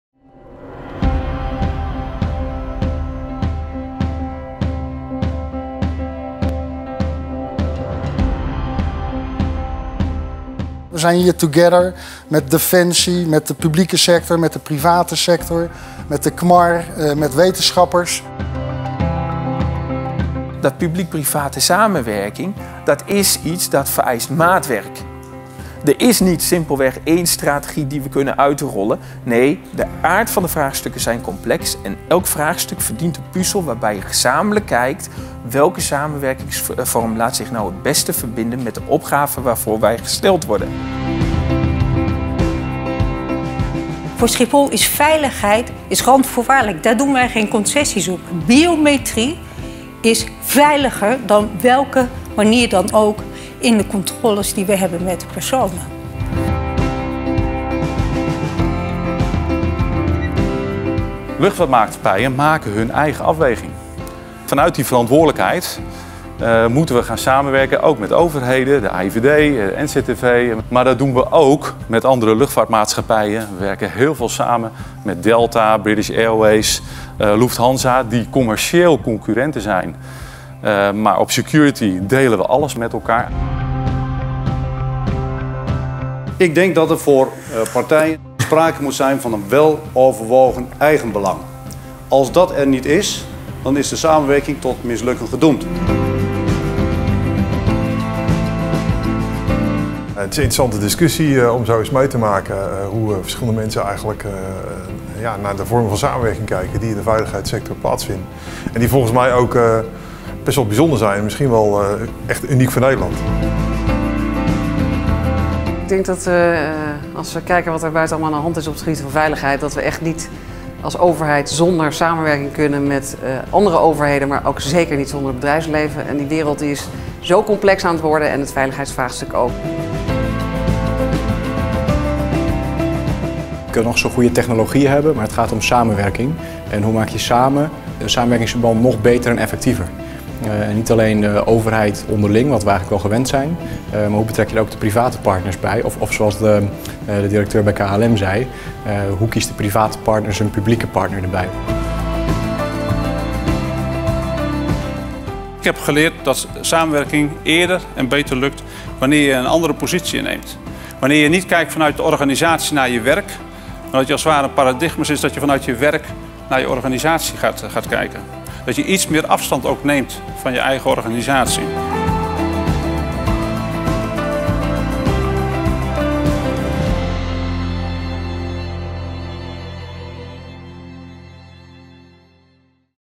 Seminar Publiek-Private Veiligheid | Nyenrode Business Universiteit
Op 22 november 2017 vond het seminar Publiek-Private Veiligheid plaats. Technologische innovaties op het gebied van veiligheid volgen elkaar in hoog tempo op. Markt en overheid moeten steeds meer en beter samenwerken. Hoe ziet de toekomst van publiek-private samenwerking eruit? Hoe kunnen we publieke, commerciële en bestuurlijke belangen constructief verenigen?

Tijdens het seminar werden deze drie invalshoeken belichten van de praktijkcase Schiphol rondom Publiek-Private Veiligheid onder leiding van Bob Hoogenboom.